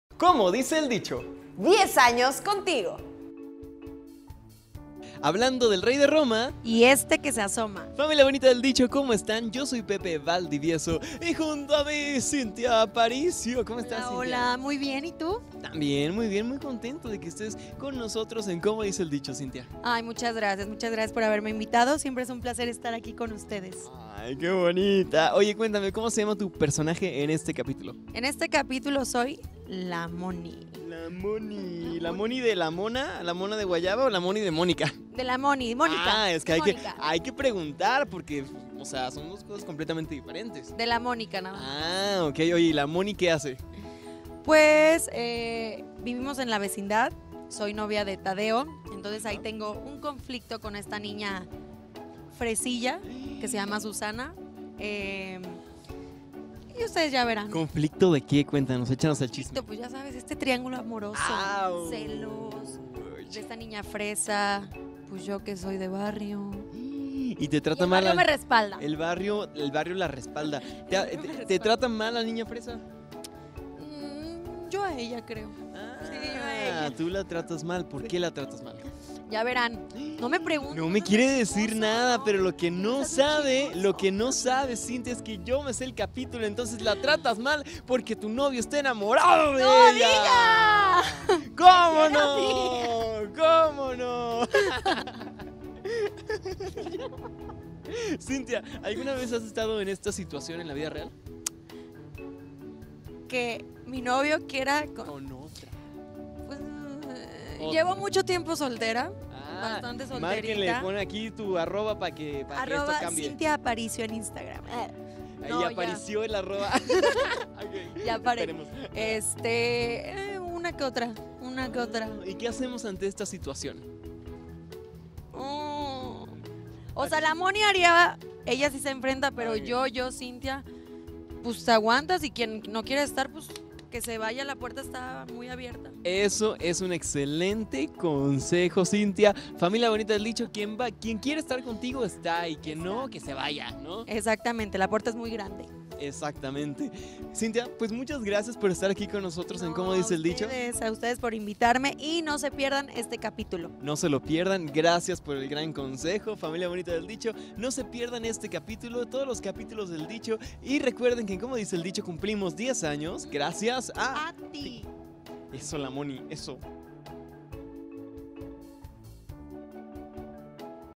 ENTREVISTA CINTHIA APARICIO COMO DICE EL DICHO
HABLANDO DEL REY DE ROMA Y ESTE QUE SE ASOMA
10 Temporada de Como dice el dicho